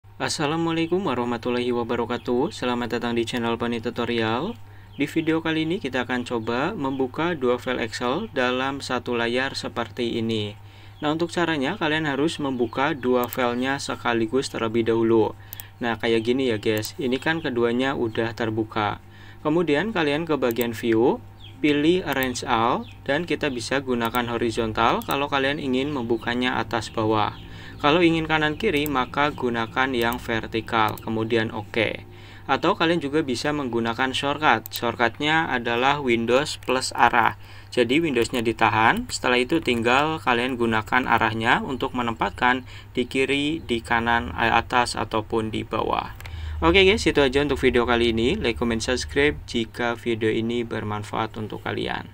Cara Menampilkan 2 File Excel Dalam Satu Layar Di Laptop/Pc
Cara Menampilkan 2 File Excel dalam Satu Layar Monitor Di Laptop/Pc

"Semoga Bermanfaat"

Seputar Chanel Kami